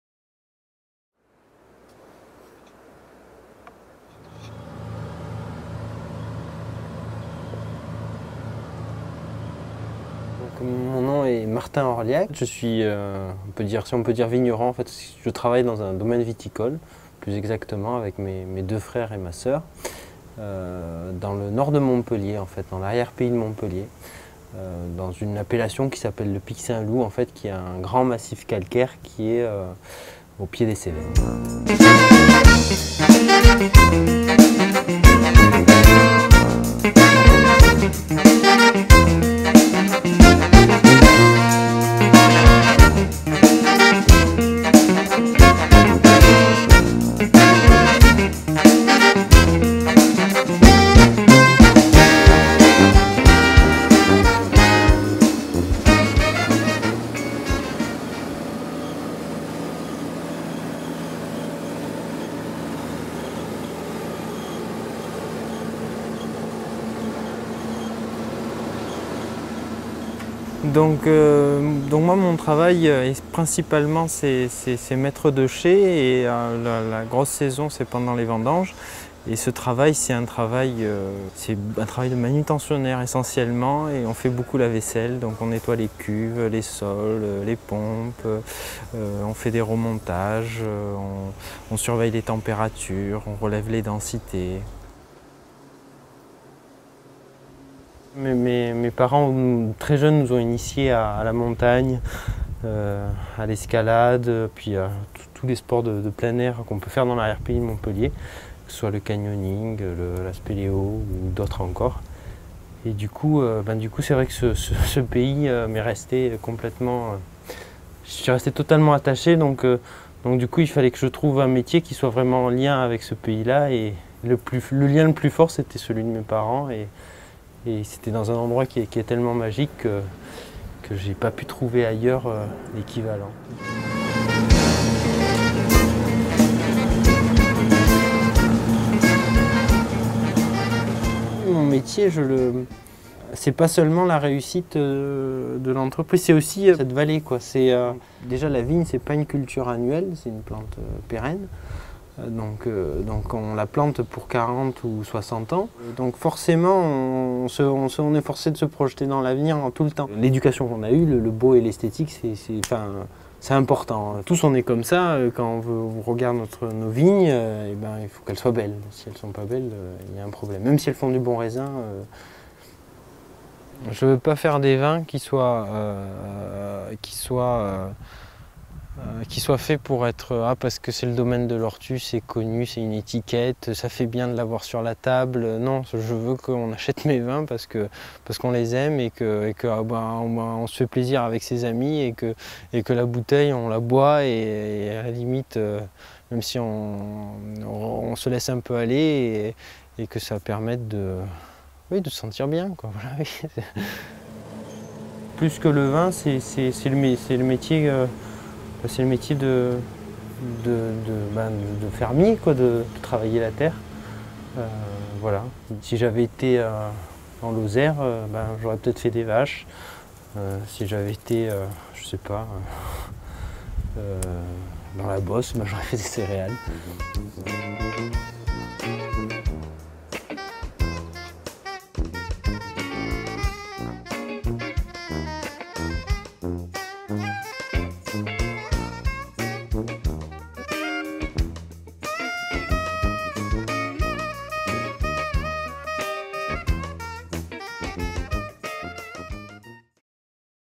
Les Hommes Livres du Languedoc - ep.3 - Martin Orliac
Hérault Tourisme 'édite'  des "livres-humains", et raconte l'histoire, l'expérience personnelle de vigneronnes et de vignerons dans leur rapport à la vigne et au vin.
Le troisième épisode met en lumière Martin Orliac du Domaine de l'Hortus : il incarne la deuxième génération de vignerons. Après des études au Japon, il nous livre sa passion de la nature et son attachement au Pic Saint-Loup.